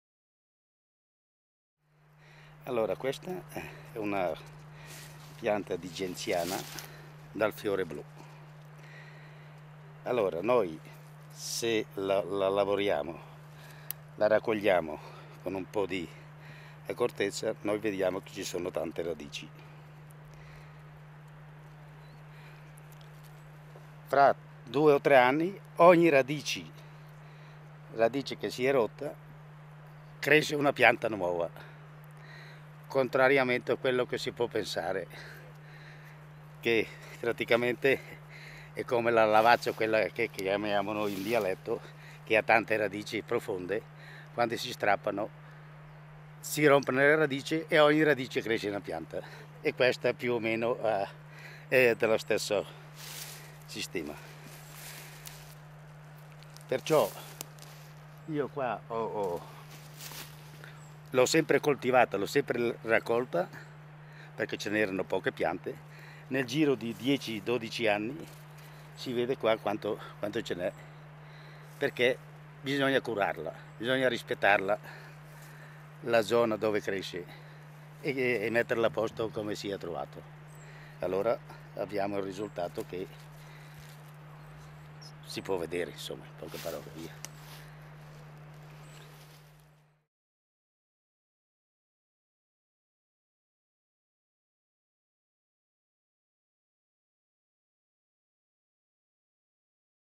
Raccolta e utilizzo della genziana in Valchiavenna
Raccolta e utilizzo della genziana in Valchiavenna - ECHI
Pratiche di raccolta e utilizzo a fini alimentari di erbe e frutti spontanei in Valchiavenna. Intervista sulla raccolta e l'utilizzo della radice di genziana (Genziana purpurea). Luciano Masolini
Isola di Madesimo (SO), Valchiavenna, 2011
Realizzato nell’ambito del progetto E.C.H.I. - Etnografie italo-svizzere per la valorizzazione del patrimonio immateriale
Archivio di Etnografia e Storia Sociale - Regione Lombardia